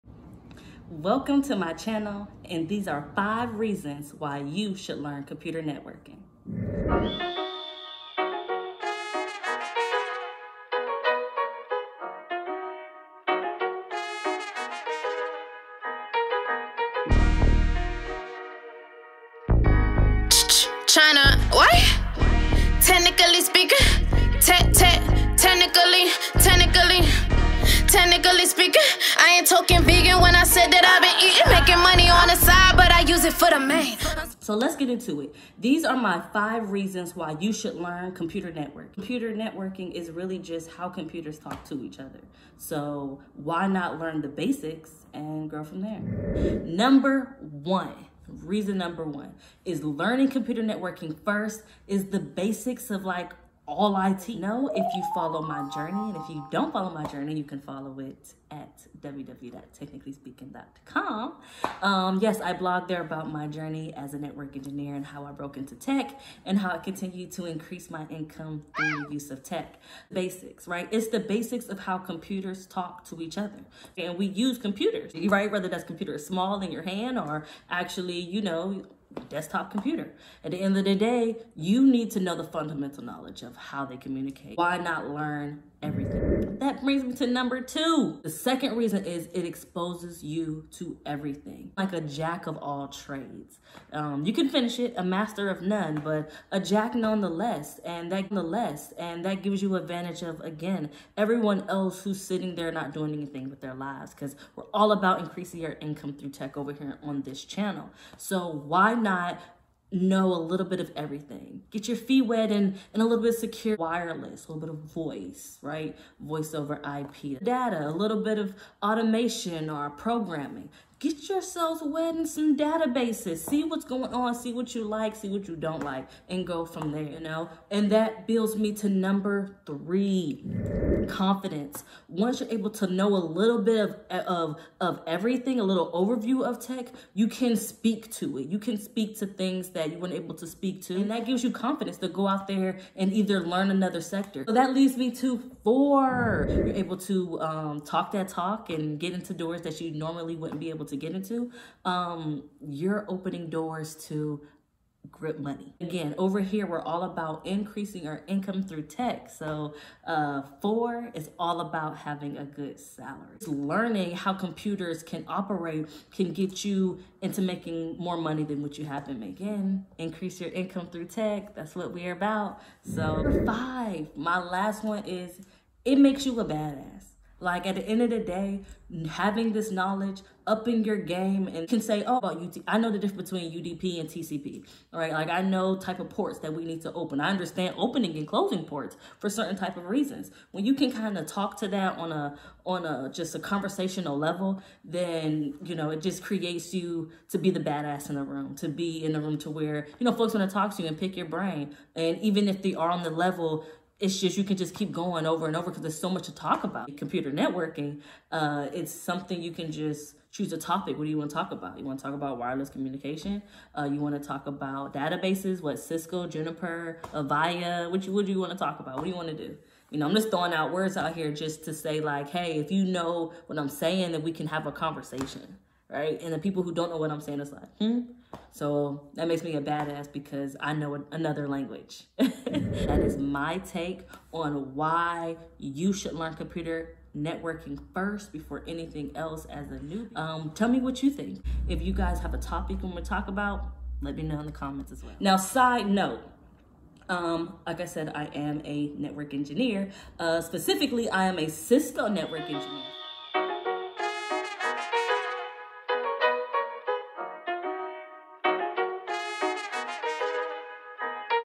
Where Do I Start In Tech?
Don't know where to start on your tech journey? Start at the beginning. Theses are my top 5 reasons why you should learn computer networking first.